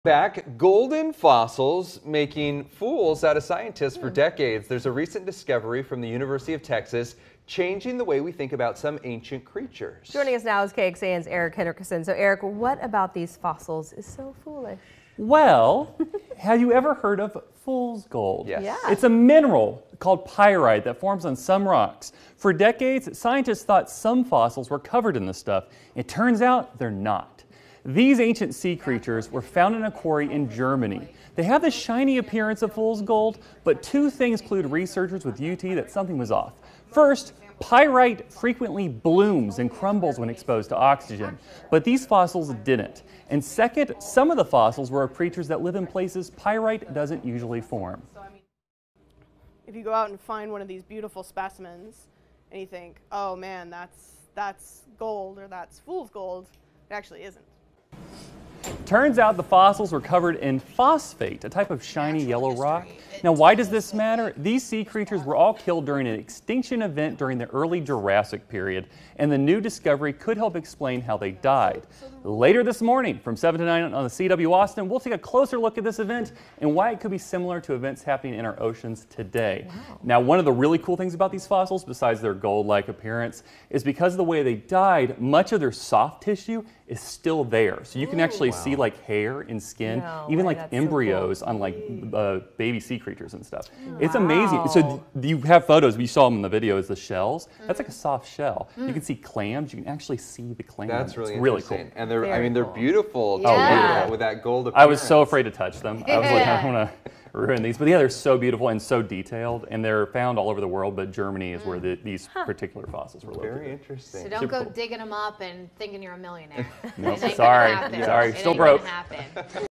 Texas researchers discover Fool's Gold fossils aren't what they thought
Fossils that have been making "fools" out of scientists for decades are getting a new look. Researchers with the University of Texas' Jackson School of Geosciences published a paper in Earth Science Reviews showing that fossils commonly thought to be coated in pyrite, also known as 'Fool's Gold", had been deceiving scientists for decades.